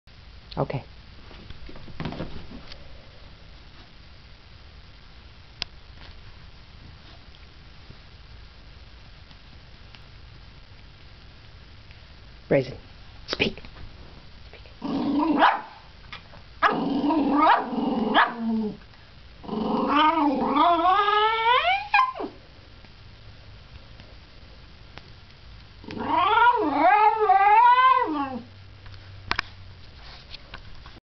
Looking into a box
Looks looks into a box ad then barks